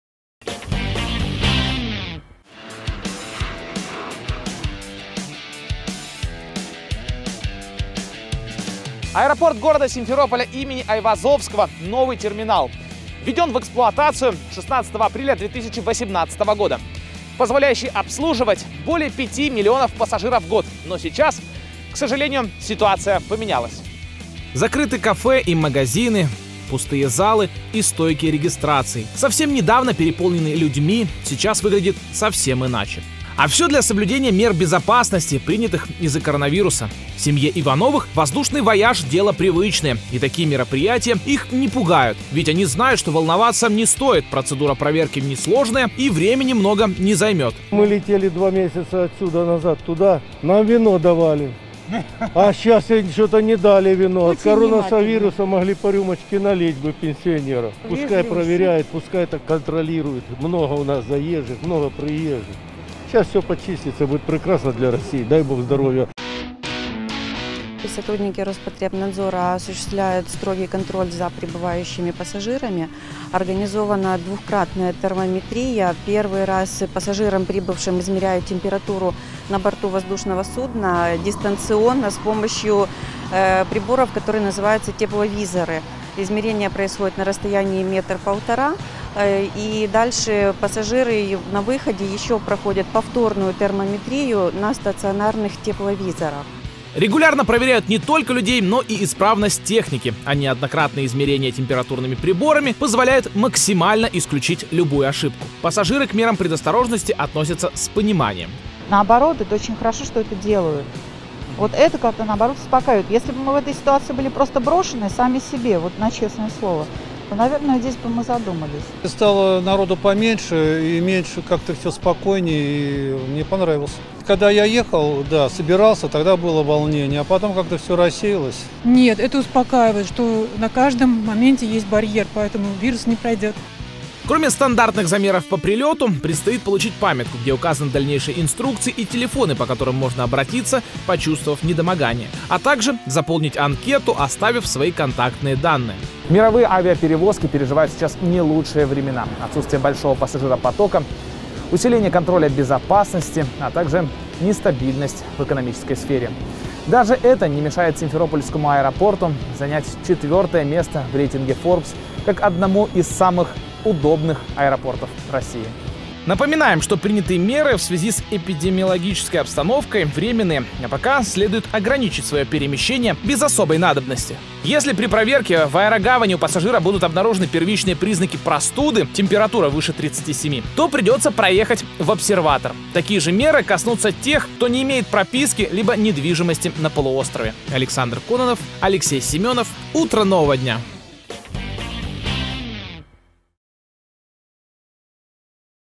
Какие меры безопасности предпринимает Аэропорт Симферополя им. Айвазовского
Закрыты кафе и магазины, пустые залы и стойки регистраций. Совсем недавно переполненный людьми, сейчас выглядит совсем иначе. А все для соблюдения мер безопасности, принятых из- за коронавируса.